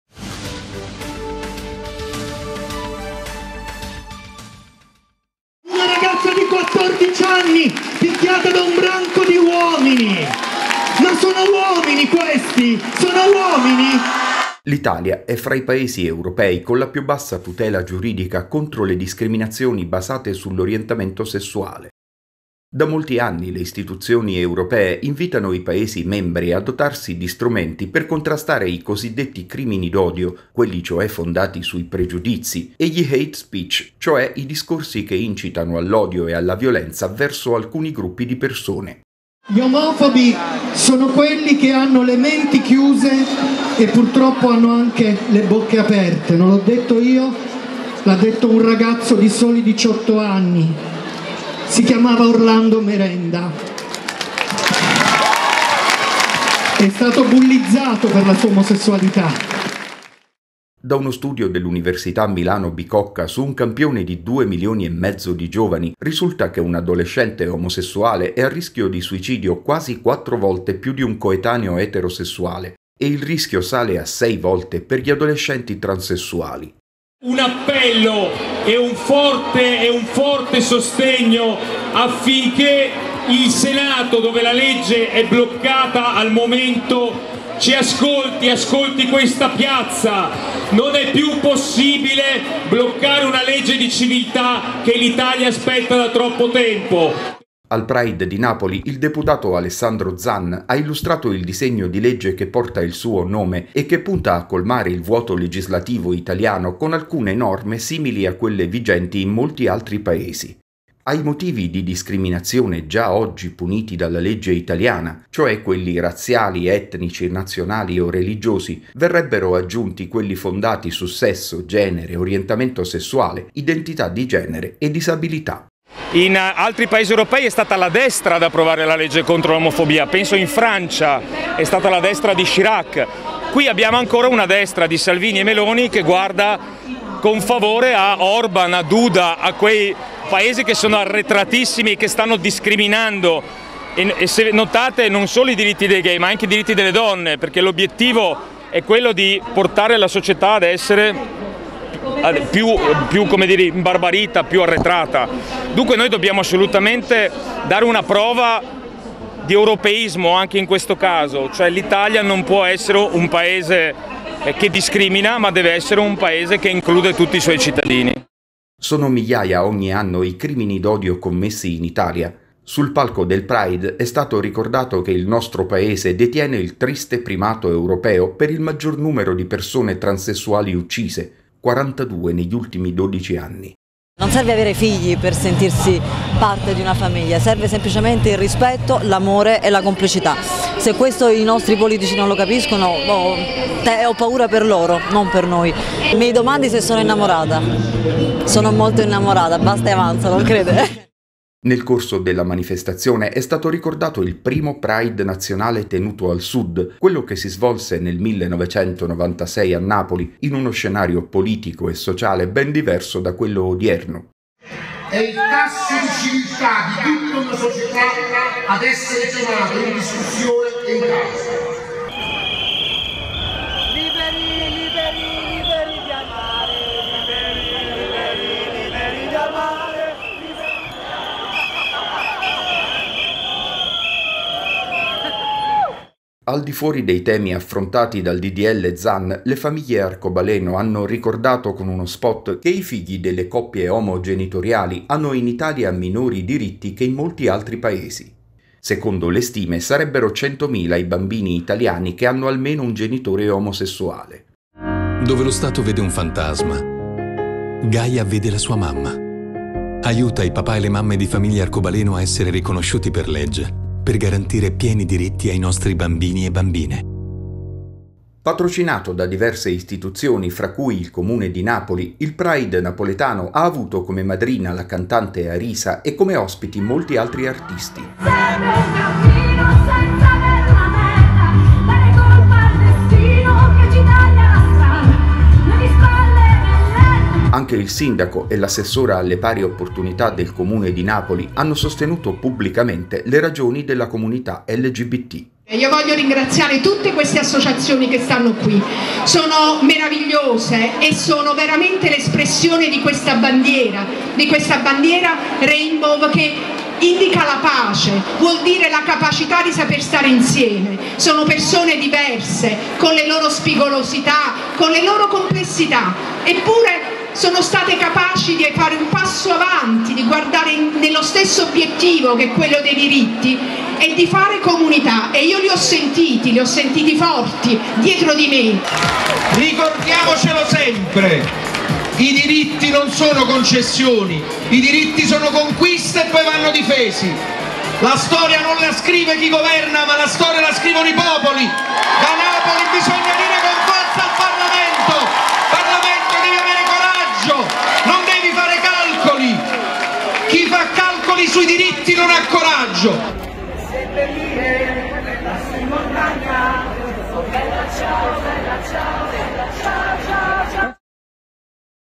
Approfondimento - Col ddl Zan l'Italia insegue l'Europa
Un breve approfondimento sul disegno di legge Zan contro i "crimini d'odio". In un confronto con gli altri Paesi d'Europa si verifica il grado di tutela garantito ai cittadini italiani contro le discriminazioni.
Secondo la graduatoria di Ilga Europe, che tiene conto di leggi, politiche, pratiche, l'Italia si colloca al 35esimo posto su 49, dopo Repubblica Ceca e Lituania.